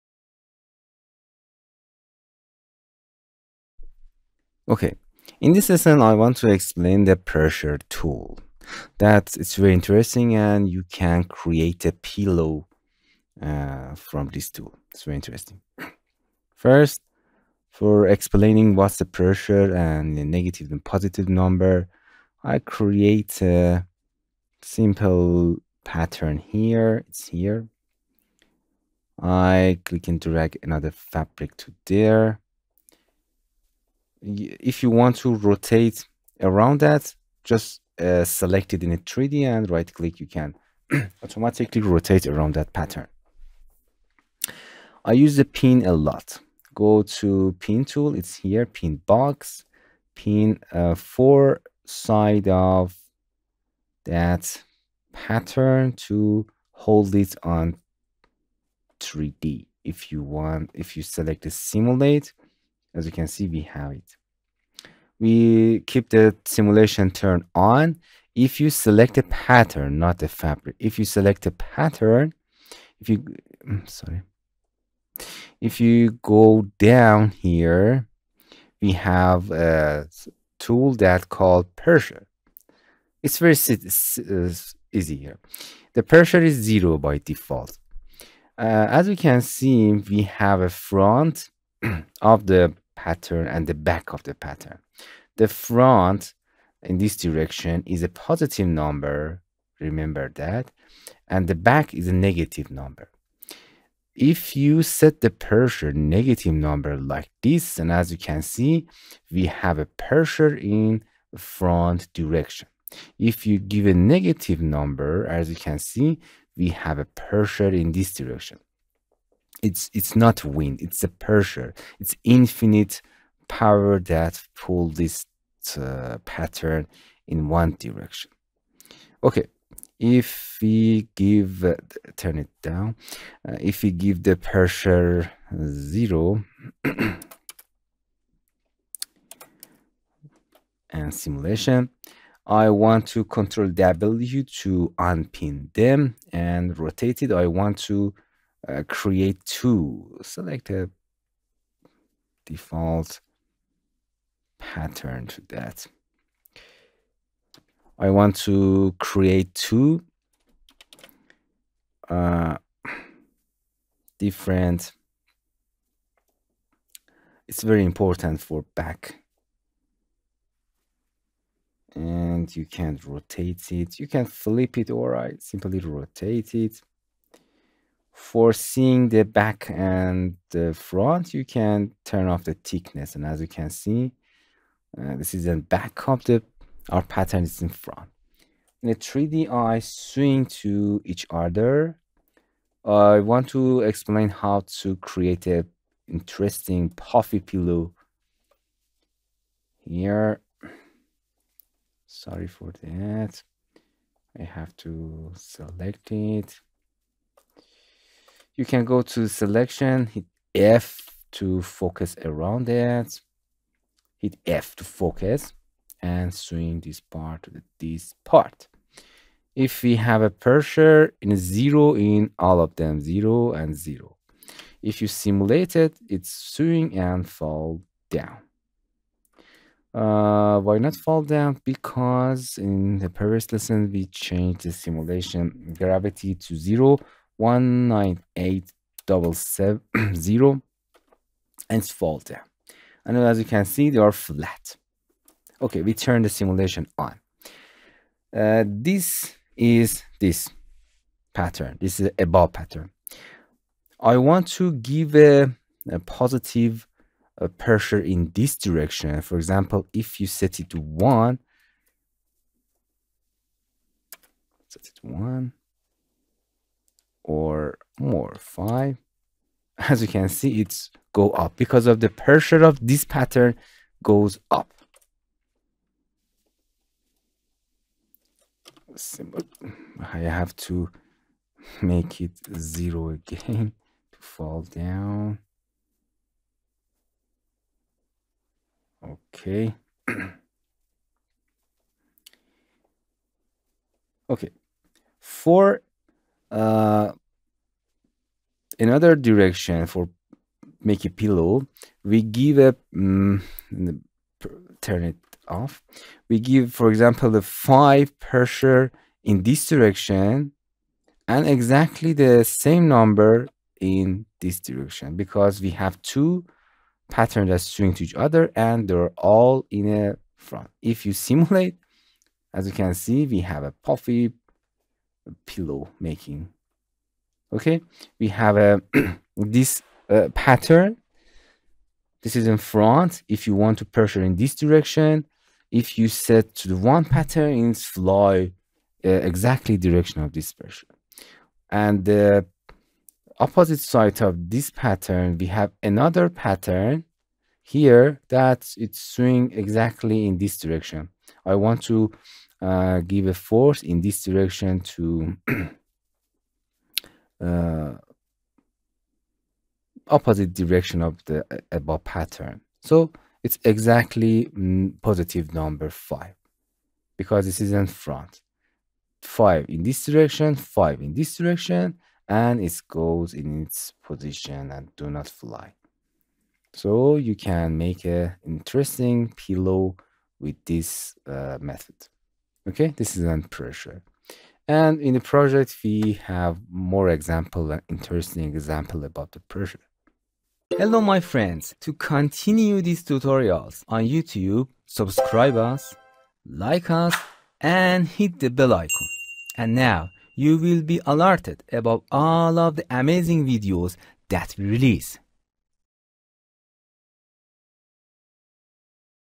0038. pressure tool in clo3D
In this lesson, we want to explain the pressure tool option.